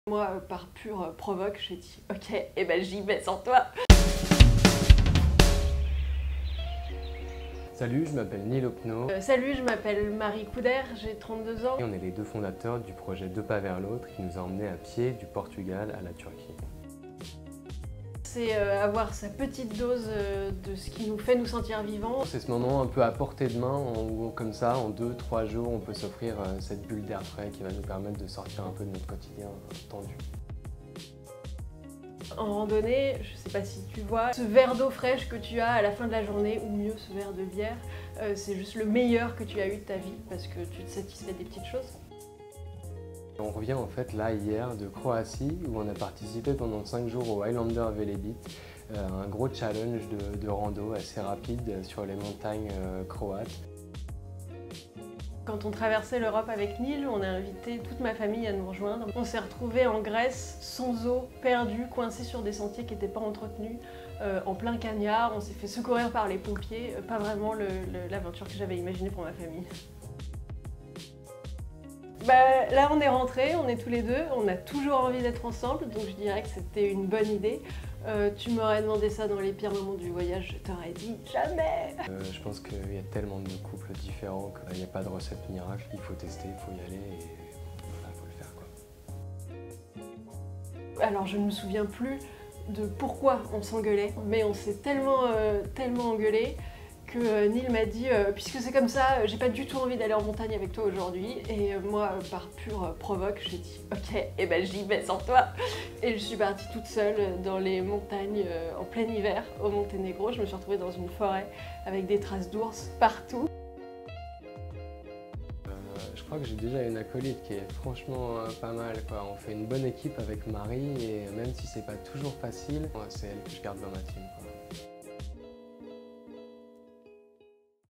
Interview Marie Couderc et Nil Hoppenot | Deux pas vers l'autre 👩‍❤️‍💋‍👨🥾
Partis en février 2018 du Portugal, les Français Marie Couderc et Nil Hoppenot ont traversé l’Europe à pied pour rallier Istanbul. Un voyage d’une traite, avec à la clé 10 000 km de randonnée extrême ultralight.  Depuis, ils continuent à vivre d'amour et d'eau fraîche... en France.